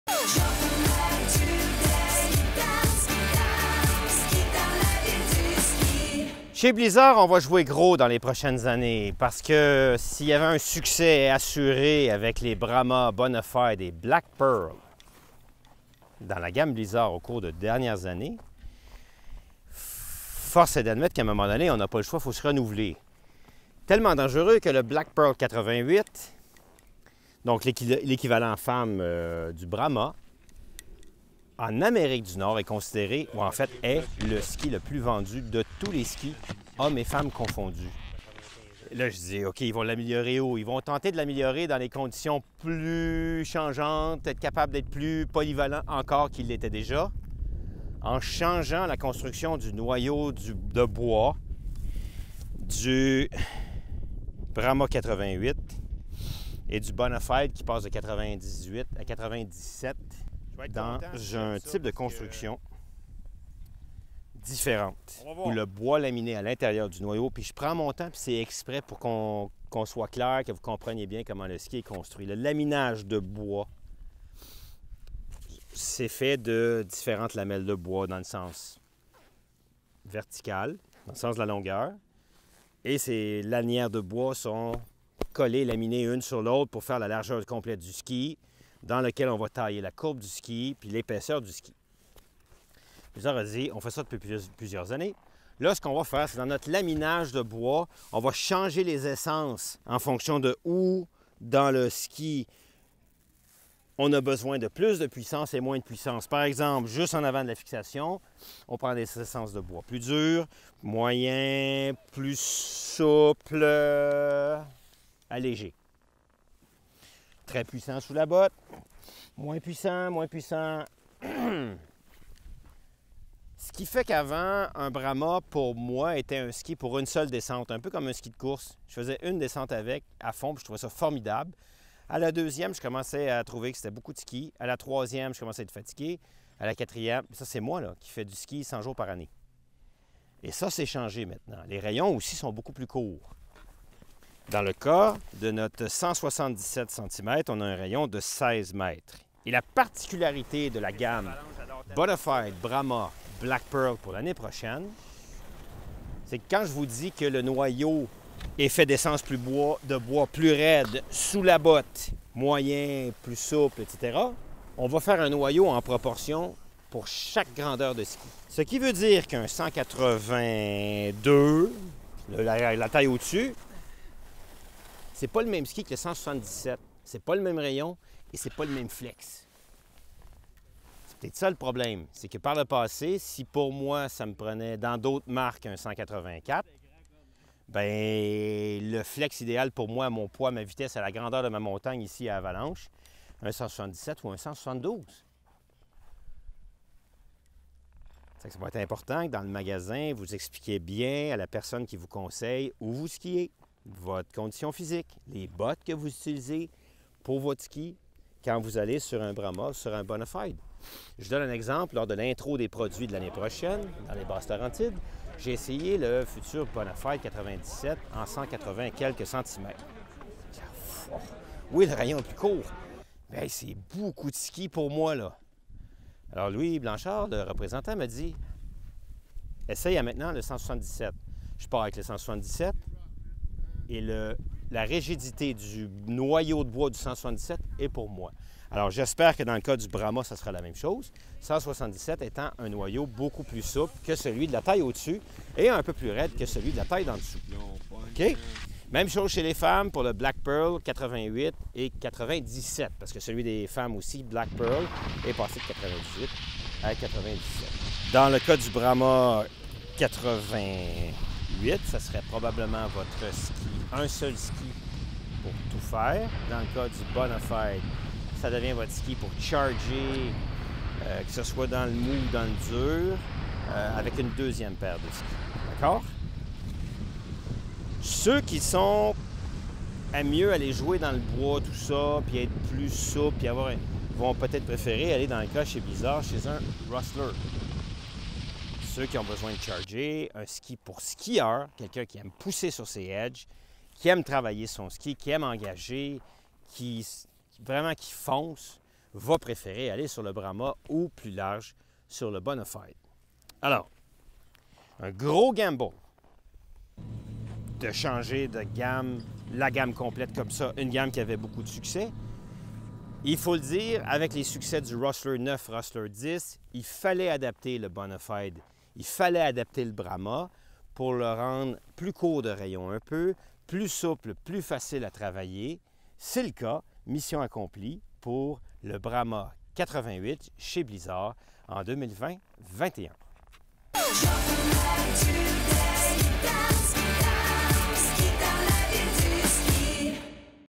SKI TEST PDS BLIZZARD Brahma 2020-21
Philippe Juneau
vous présente
dans son ski test annuel le tout nouveau Blizzard Brahma .2020-21